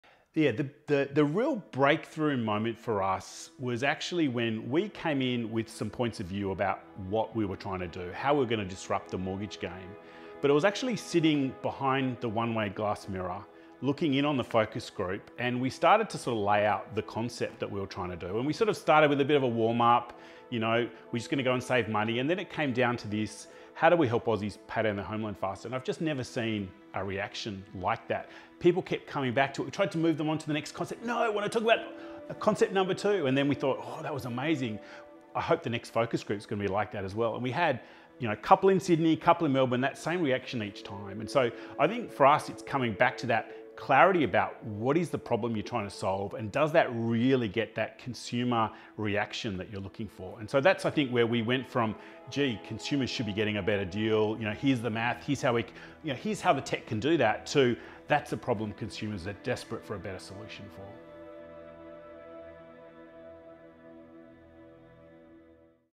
Nathan Walsh on his break-through moment with Athena Home Loans!
Hear from the Co-Founder of Athena Home Loans Nathan Walsh on his breakthrough moment with the startup business and how they are solving a real, die hard problem for millions of Aussies!